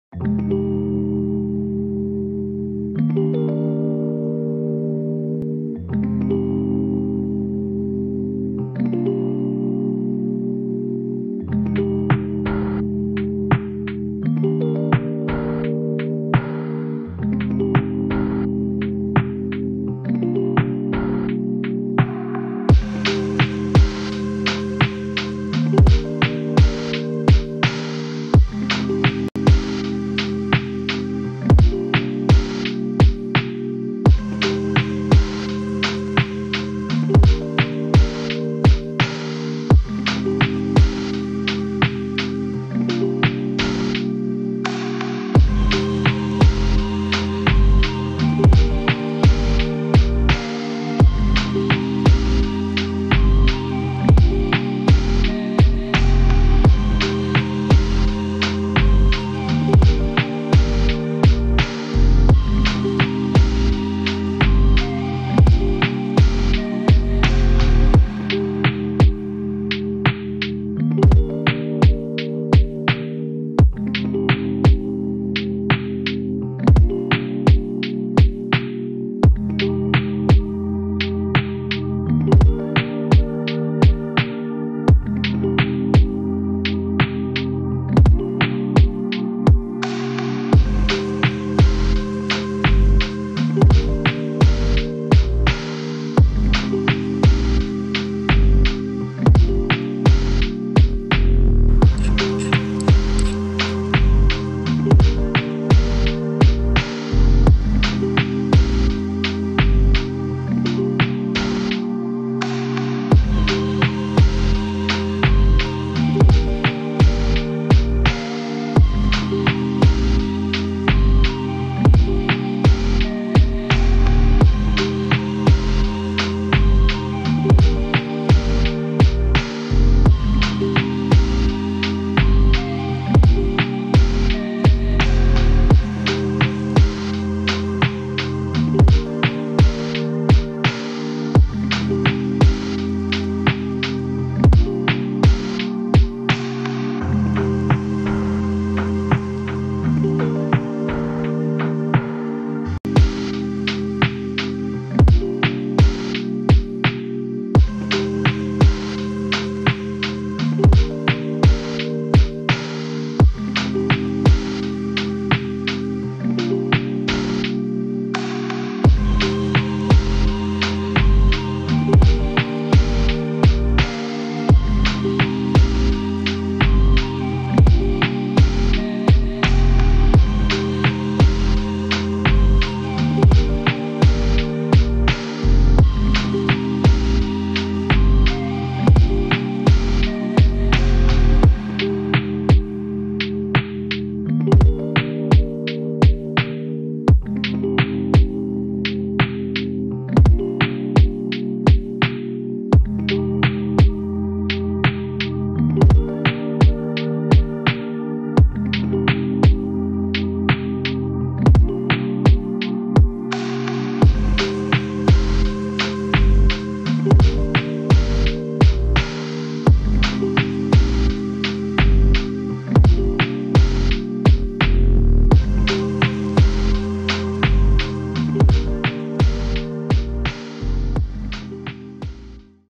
Unboxing the DJI Action 2 Extension Rod
Hey! In this video, I'm unboxing DJI Action 2 remote controller extension rod to give your first impression of the device and to see how to connect it and use it action 2 with remote.